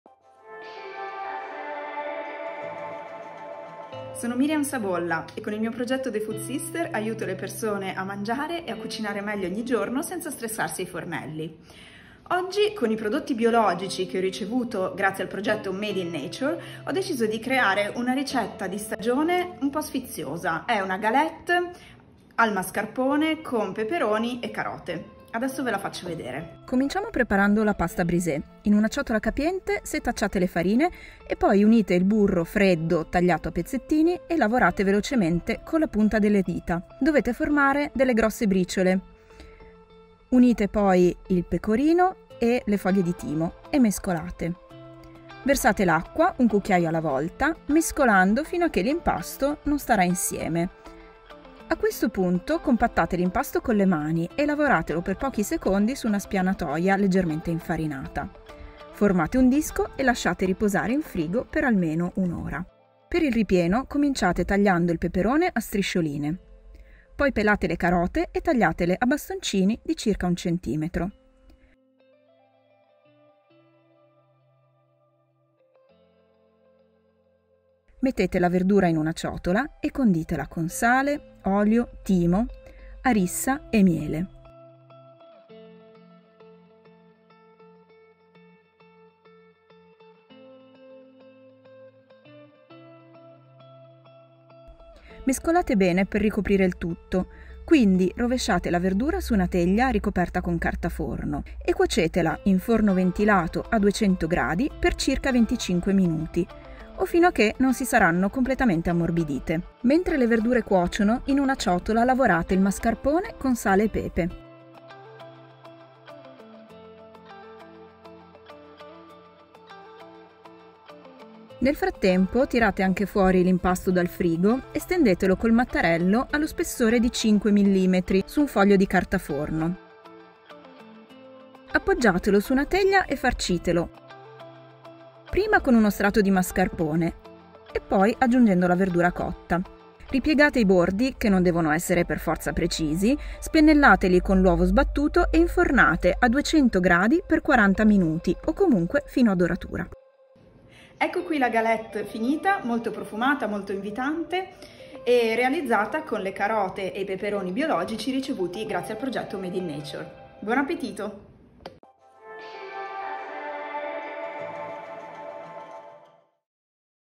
Myriam Sabolla - Galette con mascarpone e peperoni
La food influencer the food sister utilizza i prodotti biologici  per preparare una ricetta gustosa, facile e veloce!